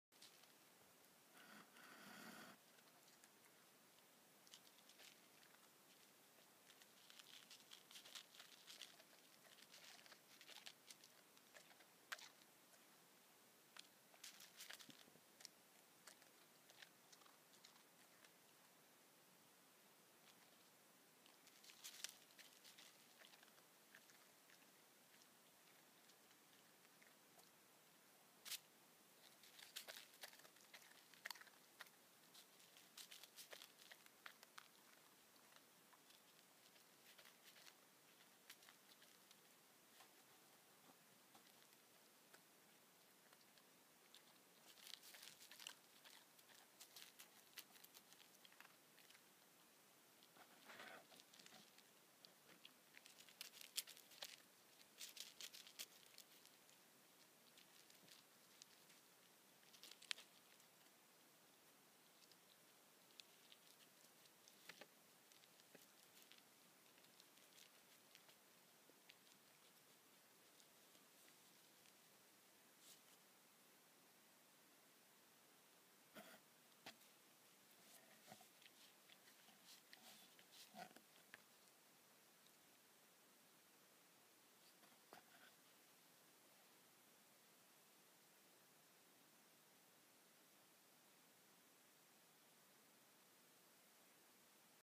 German shepherd eating granola ASMR
Today my dog is going to eat granola but it is ASMR